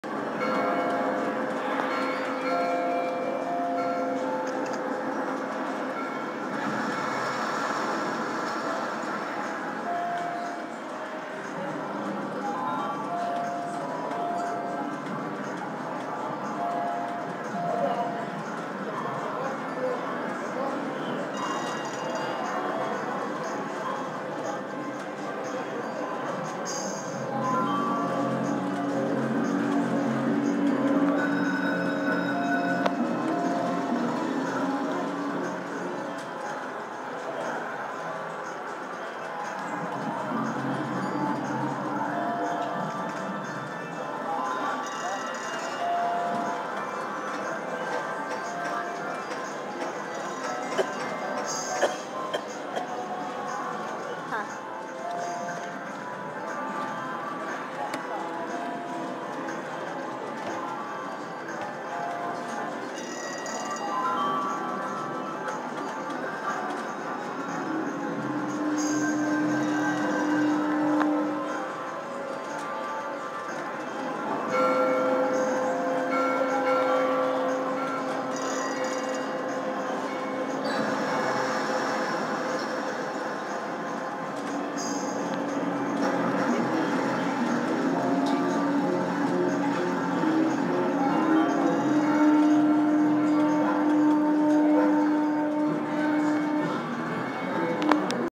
Jaws Slot Bonus
Borgata - Atlantic City - June 13, 2010
Security all around. so hard to video.